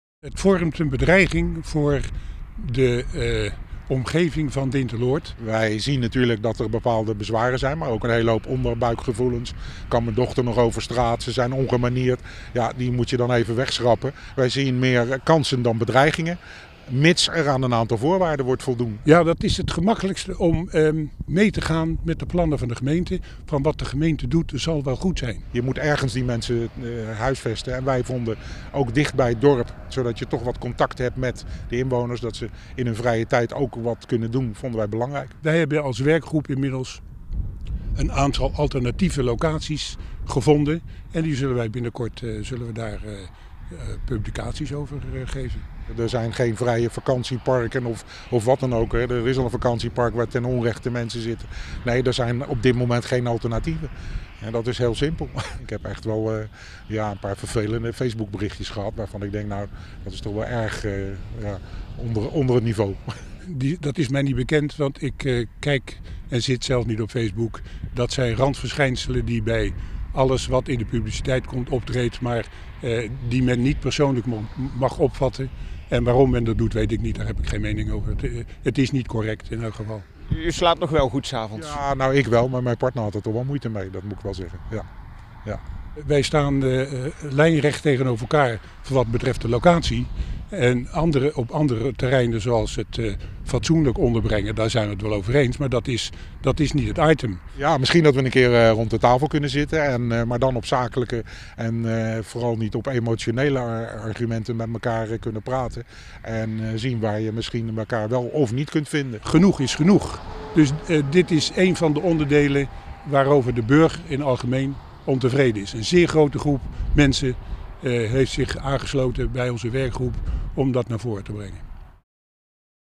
Facebook-bedreigingen en verdeeld dorp door komst 250 arbeidsmigranten: 'Genoeg is genoeg'
Een nieuw wijkje voor 250 Poolse arbeidsmigranten houdt de gemoederen flink bezig in Dinteloord. Dinsdagavond staat een volgende inloopavond voor bewoners gepland, maar aan het begin van die avond blijkt vooral dat inwoners tot op het bot zijn verdeeld. "Wij staan lijnrecht tegenover elkaar."